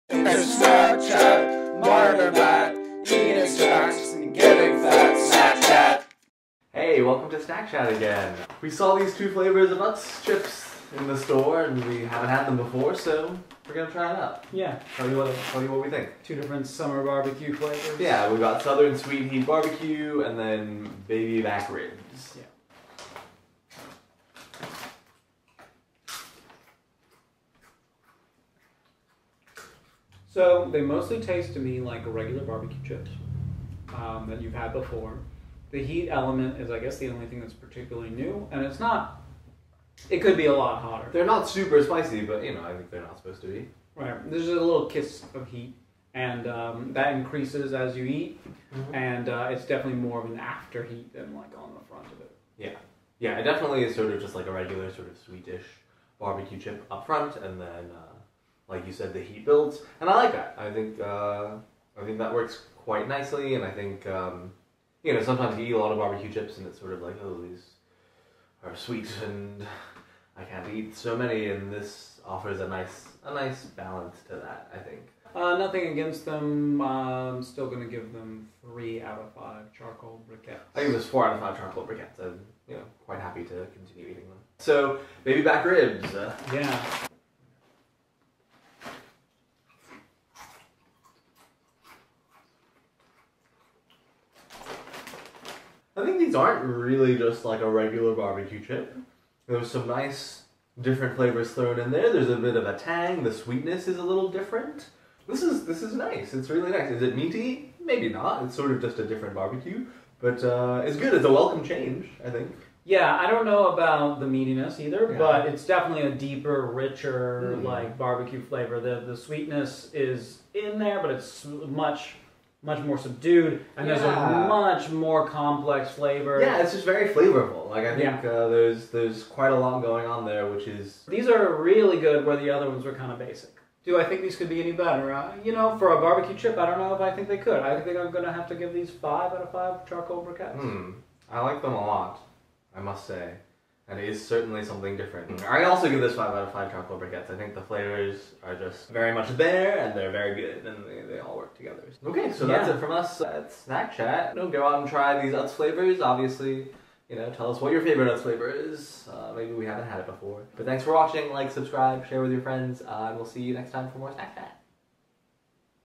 Utz Southern Sweet Heat BBQ & Baby Back Ribs
We've got a pair of new chip flavors to try here from Utz. Southern Sweet Heat BBQ, and Baby Back Ribs flavors. \

Southern Sweet Heat BBQ
Matt gave them 3/5
Myron gives them 4/5

Baby Back Ribs:
We both give them 5/5